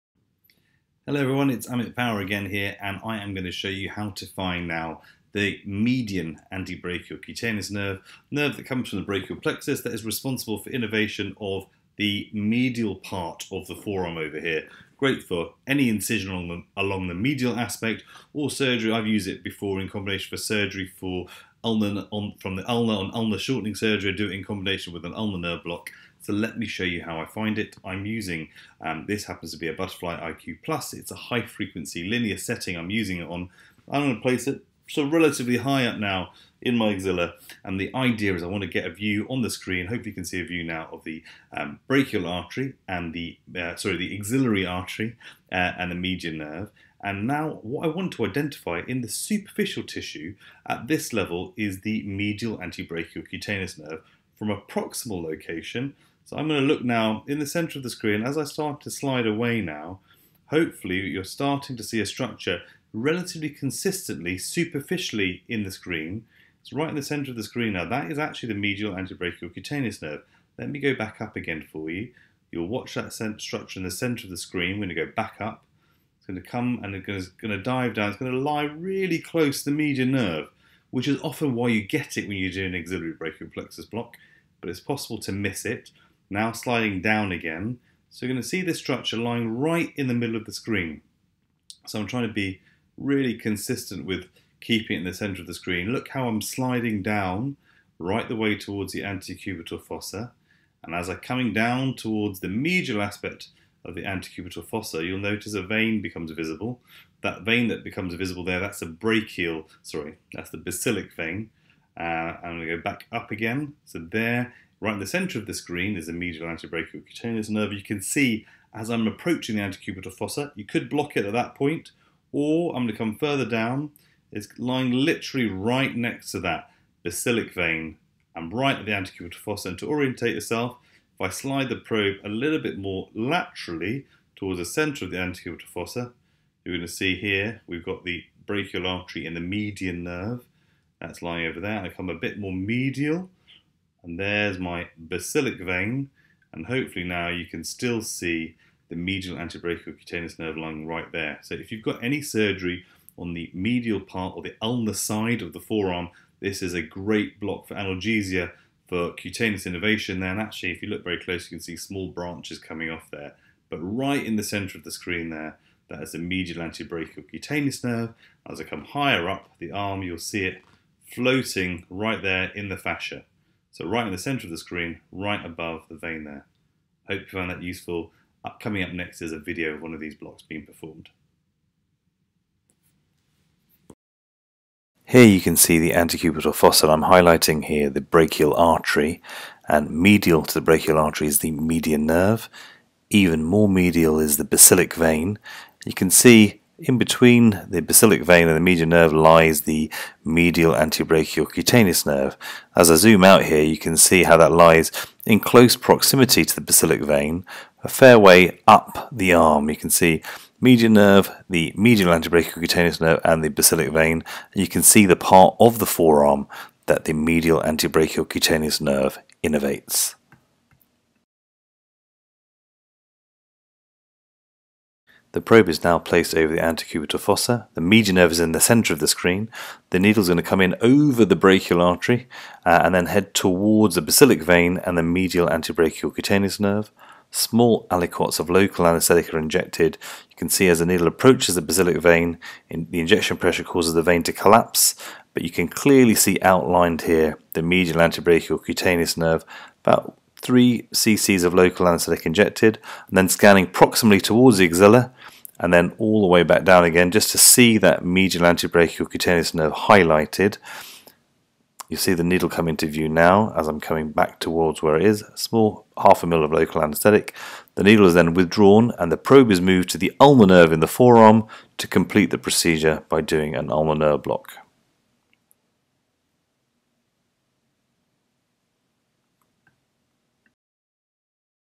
Medial Antebrachial Cutaneous Nerve block (medial cutaneous nerve of the forearm)
In This short video Dr Pawa demonstrates the scanning process for identifying the medial antebrachial cutaneous nerve (or medial cutaneous nerve of the forearm) - useful for surgery on the medial aspect of the forearm.

Demo scan performed with Butterfly iQ+
Block performed with GE LogiqE - note the orientation of the block view is the reverse of the scanning demo (medial/lateral)